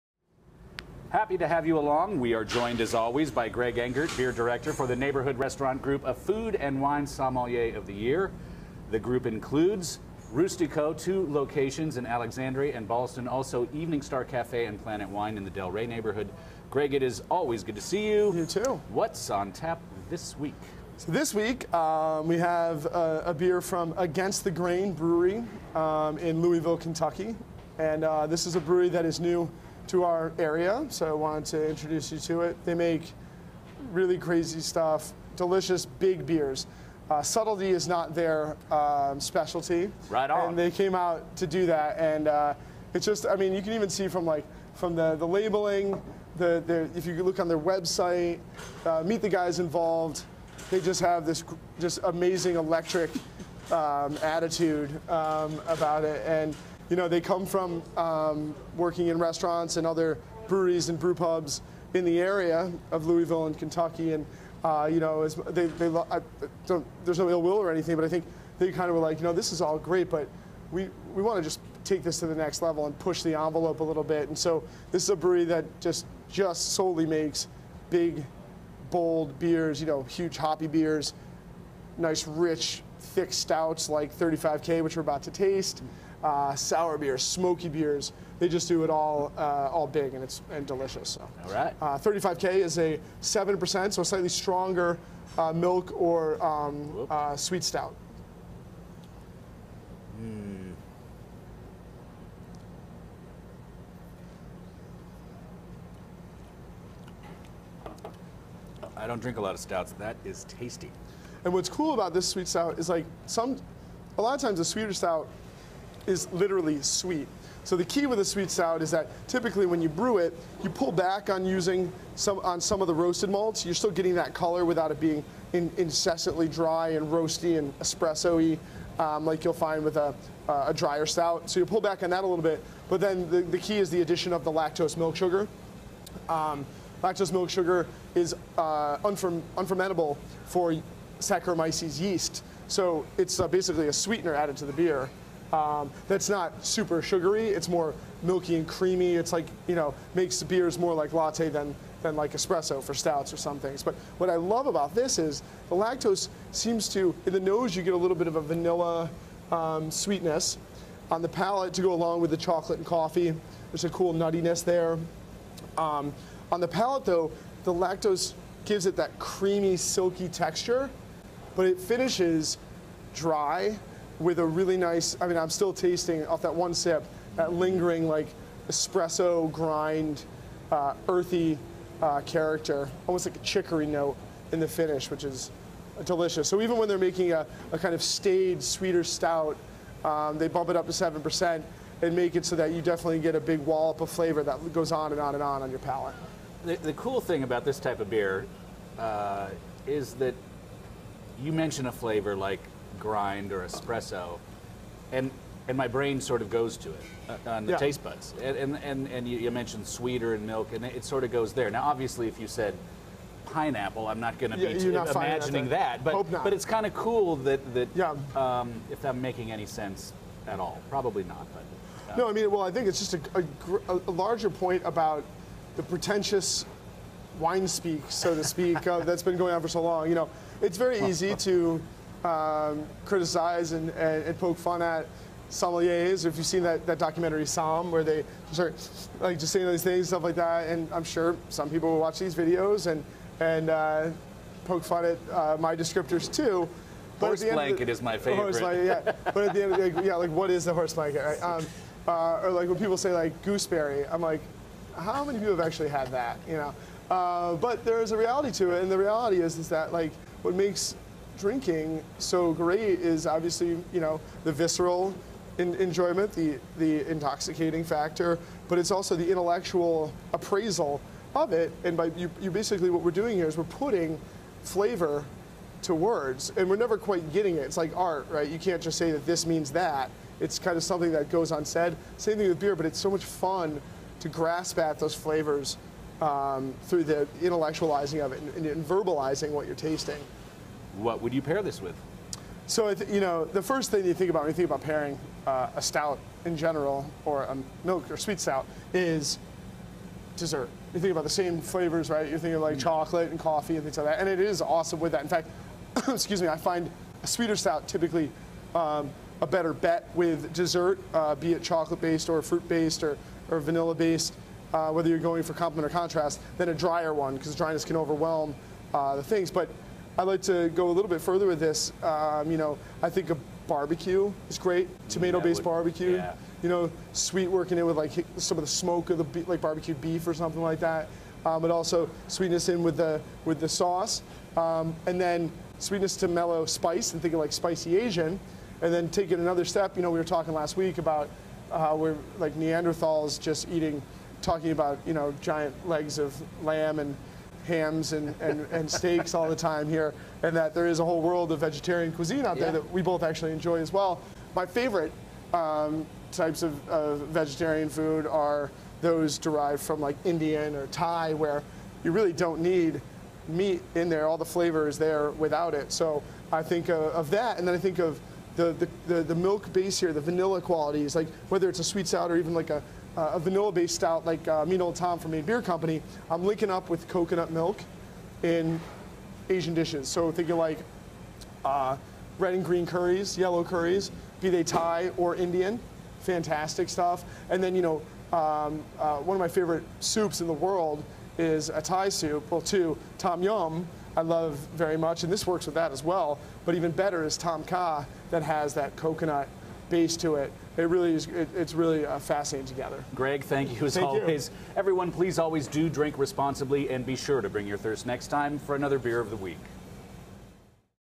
WTOP's Beer of the Week (June 28th): Against the Grain 35K
WASHINGTON -- Can something be bold and subtle at the same time? Just take a sip!

WTOP's Brennan Haselton is joined by Neighborhood Restaurant Group Beer Director and James Beard Award nominee Greg Engert to talk about Against the Grain 35K for the latest edition of WTOP's Beer of the Week.